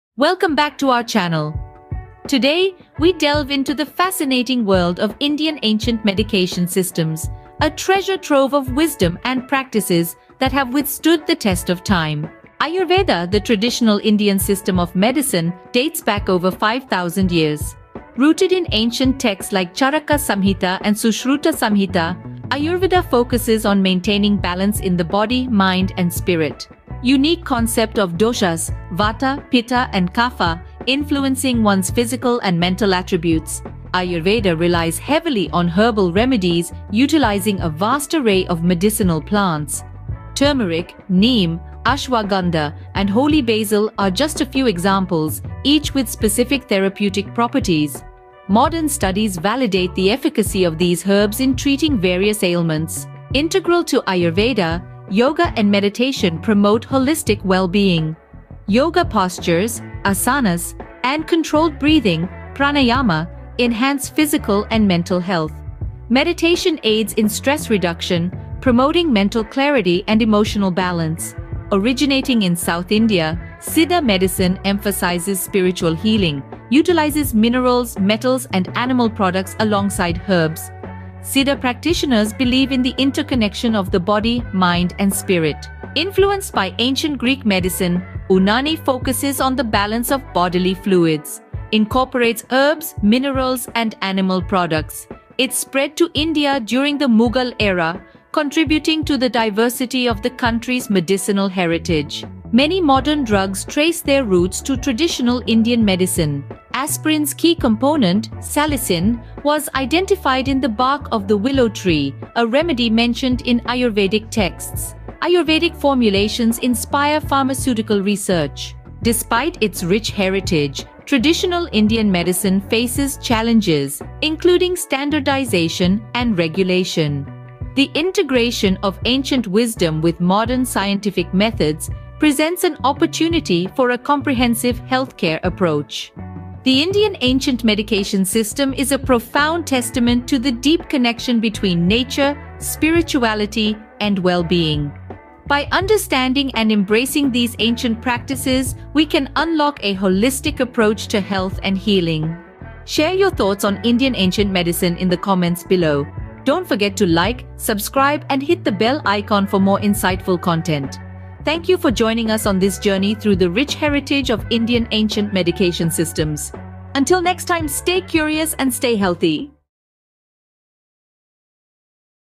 The Rich Heritage of Indian Ancient Medication System
Video Description :~
In this captivating video, we embark on a journey to uncover the rich heritage of Indian ancient medication systems and delve into their profound impact on modern medicine. Join us as we explore the roots of Ayurveda, Siddha, and Unani, and discover how these traditional healing practices blend seamlessly with modern science.

Intrigued by the ancient wisdom of these medication systems? We'll discuss how practitioners have adapted these time-honored practices to suit our contemporary lifestyles and address today's health challenges.

Unlock the secrets of Indian medicinal herbs, natural remedies, and holistic approaches that have stood the test of time. Our expert guests will shed light on the incredible therapeutic properties of these remedies, and how they can complement and enhance modern medical treatments.

From the mind-body balance of Ayurveda to the potent herbs of Siddha and the holistic healing principles of Unani, this video will provide an in-depth understanding of the Indian ancient medication systems. Discover how they continue to be relevant, sought after, and integrated into modern healthcare practices around the world.

Prepare to be enlightened, amazed, and inspired by this captivating exploration of India's ancient medication systems. Join us as we embark on a journey that will forever change your perspective on healing and medicine.

OUTLINE:

00:00:00 Introduction to Indian Ancient Medication Systems
00:00:12 Ayurveda - The Traditional Indian System of Medicine
00:00:33 Herbal Remedies in Ayurveda
00:00:53 Yoga and Meditation in Ayurveda
00:01:11 Siddha Medicine - Spiritual Healing from South India
00:01:25 Unani Medicine - Balance of Bodily Fluids
00:01:42 Modern Drugs and Traditional Indian Medicine
00:01:58 Challenges and Opportunities in Traditional Indian Medicine
00:02:16 The Deep Connection between Nature, Spirituality, and Well-being
00:02:32 Conclusion and Call to Action